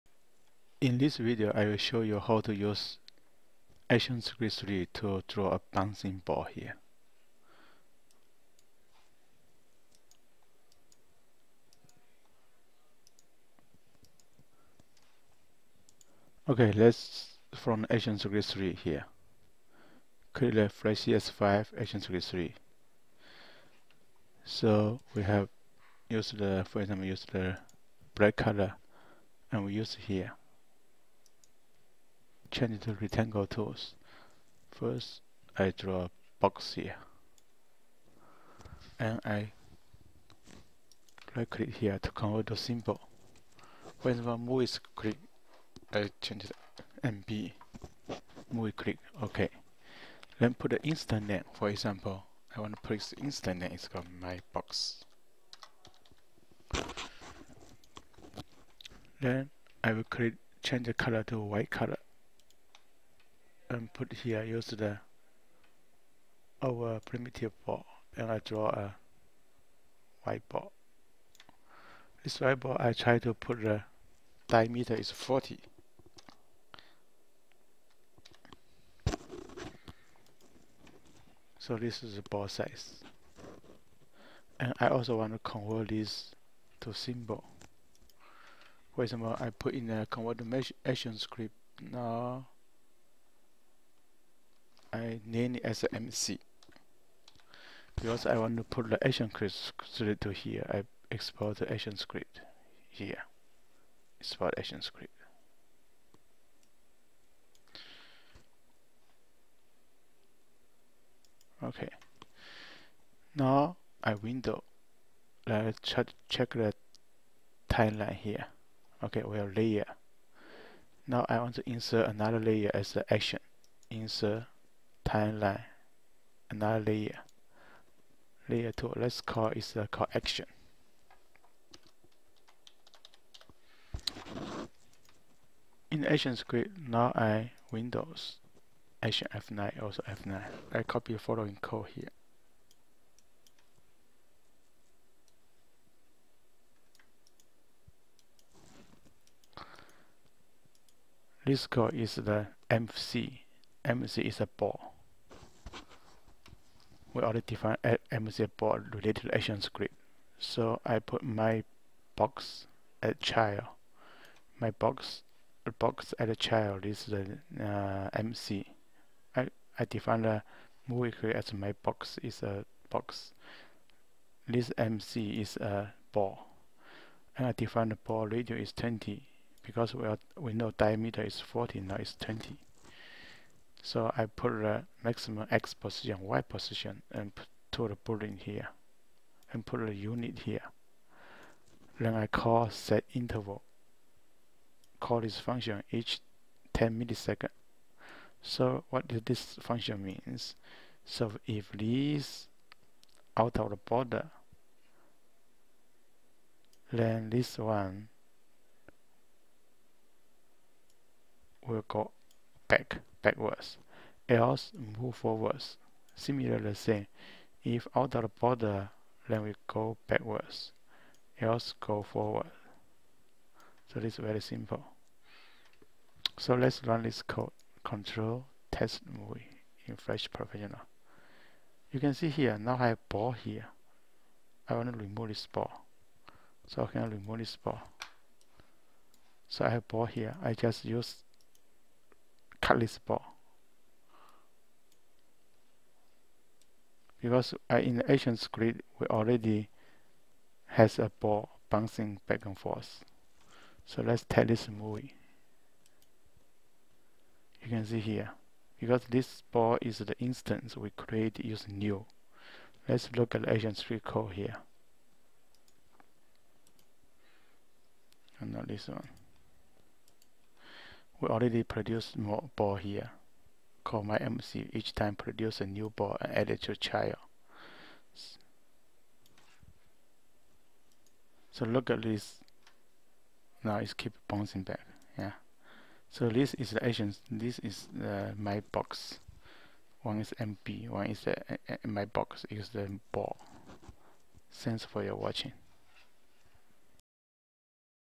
Actionscript 3 (as3) demo: design a bouncing ball animation in flash
To design a bouncing ball  in flash using ActionScript 3.0,

1) Start Flash CS5, click new -- actionscript 3.0, create a rectangle at Center using Rectangle Tool, convert it  to MovieClip with name mb  and the instance name: mybox
2) Create a ball and convert it to a MovieClip with name mc, in linkage select check box: Export to ActionScriot and Export to frame 1, name class as mc.
3) Create action layer, Press F9,  ActionScript .